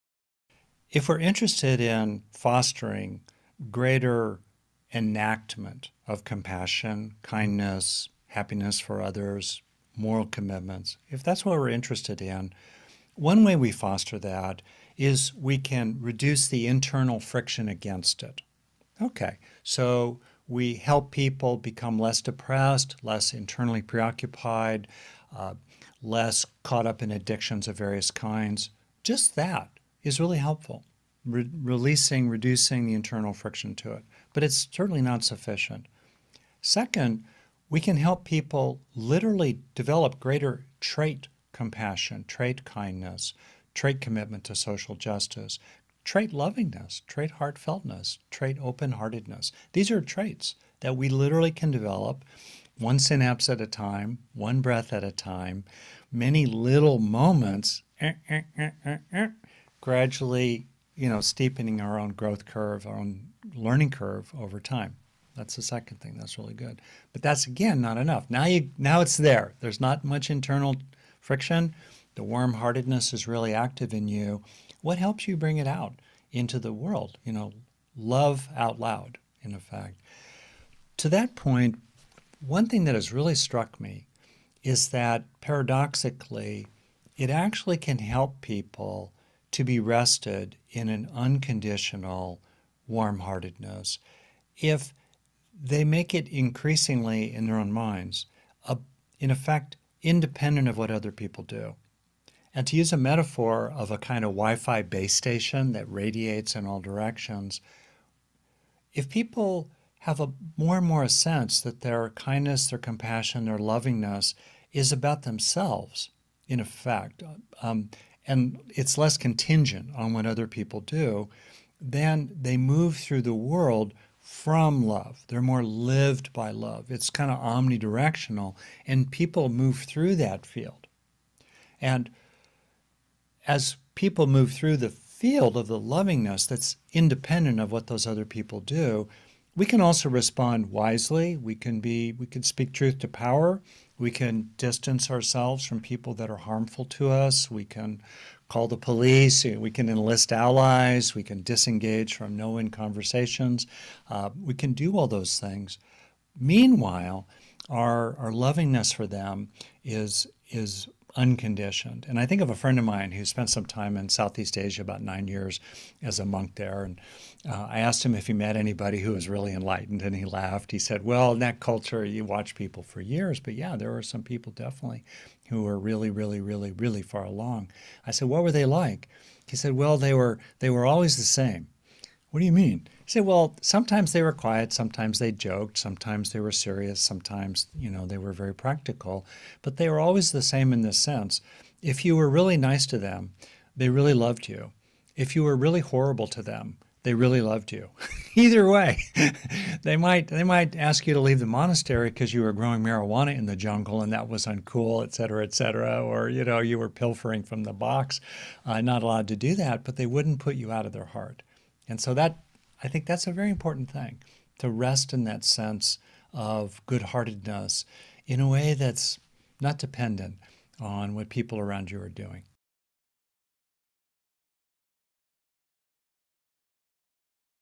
How to Have Compassion for Others No Matter What They Do - Interview with Dr. Rick Hanson
How can we develop a more loving and compassionate nature that we can extend to others - no matter what they may do?

This clip was taken from the "Creating a Compassionate World" interview series hosted by Professor Paul Gilbert, OBE.